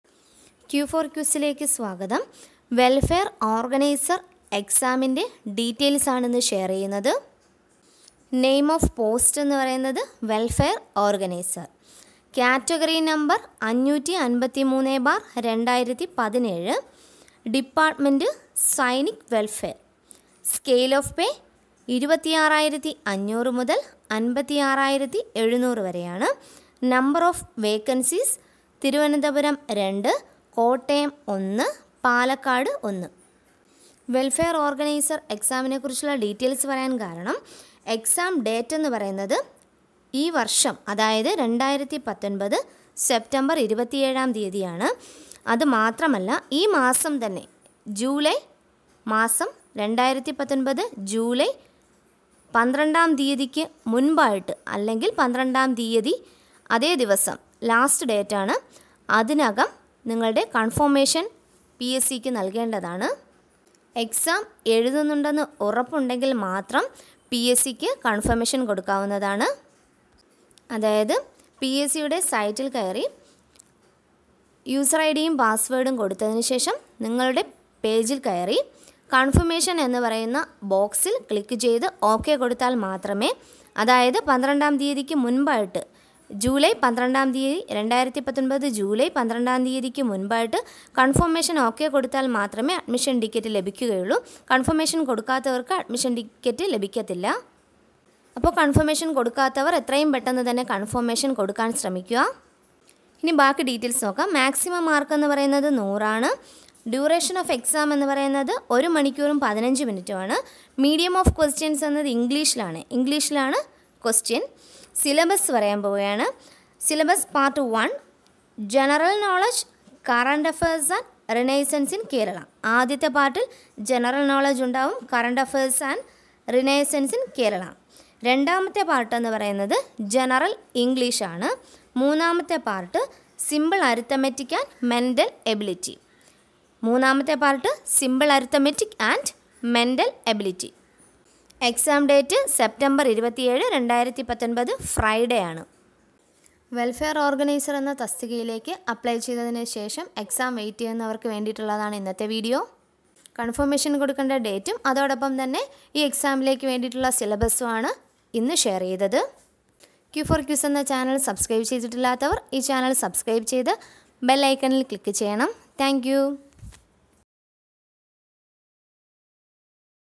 PSC :WELFARE ORGANISER EXAM :Confirmation Date and Syllabus
Psc നടത്തുന്ന welfare organiser exam നെക്കുറിച്ചുള്ള വിവരങ്ങളാണ് ഈ ഭാഗത്തിൽ ഉൾപ്പെടുത്തിയിട്ടുള്ളത്. എക്സാം തീയതിയും  സിലബസും ആണ്  ഈ ഭാഗത്തിൽ പറഞ്ഞിട്ടുള്ളത്